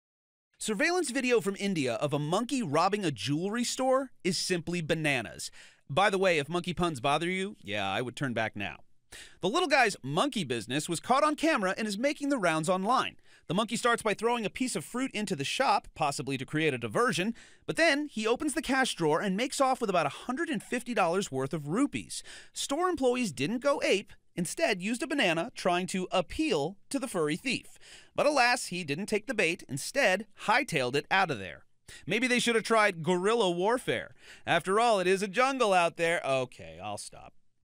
Video: Monkey caught on video robbing jewelry store in India
This video is bananas. A monkey was caught on security camera robbing a jewelry store in India. CNN has the story.